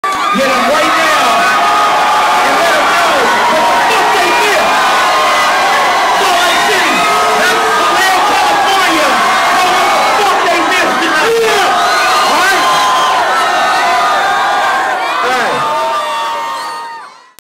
MAC DRE TRIBUTE CONCERT - SALT LAKE CITY PART 1
FILMED OFF OF MY IPHONE.
SALT LAKE CITY SHOWED UP.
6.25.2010